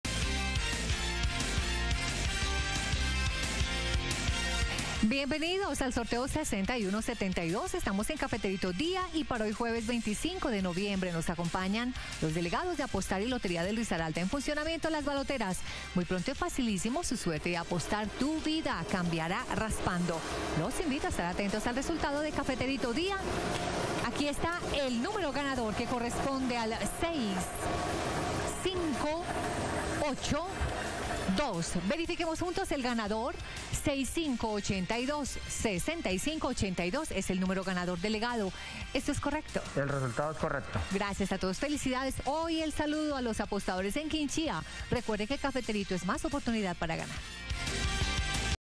Resultado EL CAFETERITO TARDE del jueves 25 de noviembre de 2021 ✅🥇🔥💰
Hola buenas tardes, te presentamos los resultados de las loterías y chances en Colombia, vamos con el Resultados del Chance Cafeterito Tarde del jueves 25 de noviembre de 2021.

cafeterito tarde
cafeterito dia